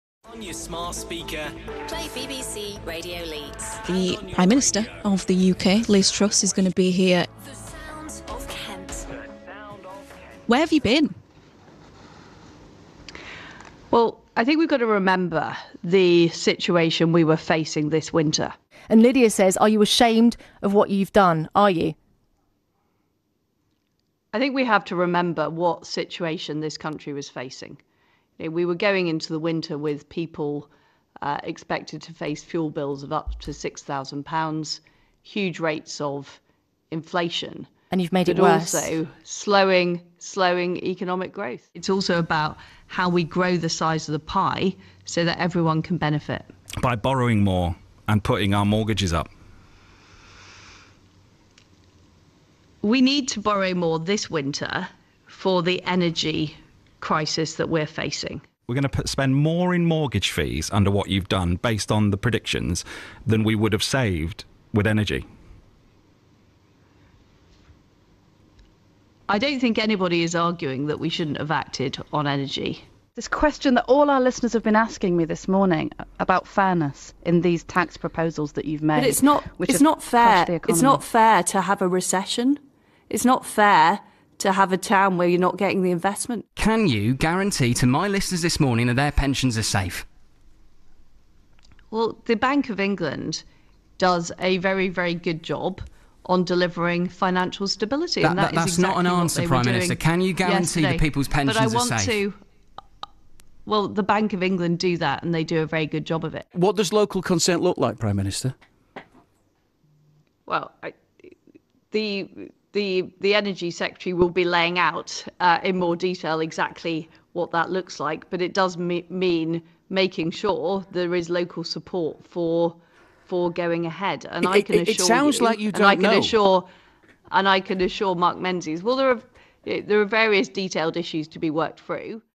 トラス英首相、BBCの地方ラジオ各局に出演 経済政策に厳しい質問相次ぐ
イギリスではこのほど、トラス英政権が450億ポンド規模の減税策を発表するや、ポンドが急落、国際通貨基金（IMF）が政策を警告、英中央銀行が市場安定化のために国債購入を発表する事態が相次いでいる。これについてリズ・トラス首相の発言が注目される中、首相は29日、複数のBBC地方ラジオ局に出演。そこで相次いだ厳しい質問と、それに対するトラス氏の反応が話題になった。

トラス氏は与党・保守党の党首選で一貫して、大規模減税を公約し続けた末、首相に就任した。クワジ・クワーテング財務相は23日、大幅な政府借り入れを前提とした450億ポンド規模の減税政策「ミニ・バジェット（小さな予算）」を発表。所得税や住宅購入の際の印紙税を減らすほか、法人税の増税計画も廃止する。これにより、経済成長に弾みをつけたいとしている。

ただしこれを警戒する市場では英ポンドが大幅に下落したほか、英国債や株式にも影響が出て、政府の借り入れコストが大幅に上がっているほか、一般市民の住宅ローンなども借り入れコストが上昇している。27日には国際通貨（IMF）が「不平等を加速させる」と異例の警告を発し、英イングランド銀行（中央銀行）が28日、市場安定化のために国債を購入すると発表した。

こうした中でトラス氏は29日、首相官邸からイングランド各地の地方ラジオ局を相手に、7分間のインタビューを計8回、1時間かけて相次ぎ行った。これは秋の党大会前に時の総理大臣が行う慣例行事で、今年の保守党党大会は10月2日からバーミンガムで始まる。